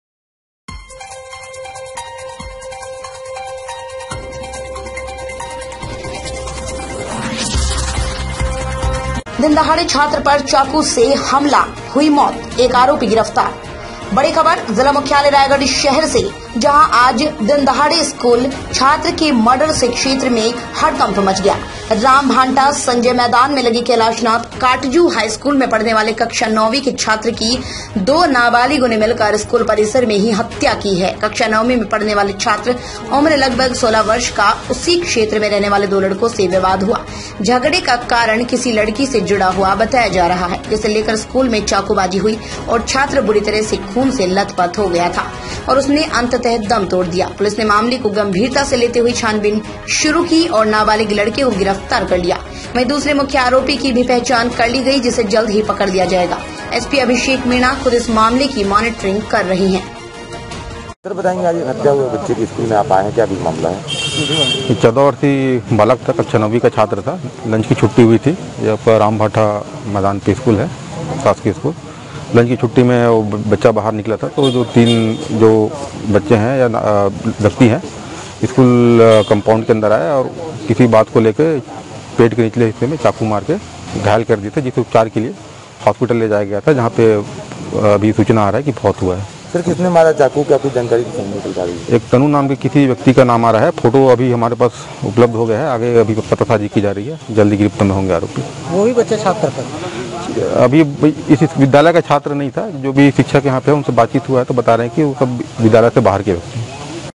रायगढ़ :दिनदहाड़े छात्र पर चाकू से हमला हुई मौत, एक आरोपी गिरफ्तार
रायगढ़ #@जौहरछत्तीसगढ़
दिनदहाड़े छात्र पर चाकू से हमला हुई मौत, एक आरोपी गिरफ्तार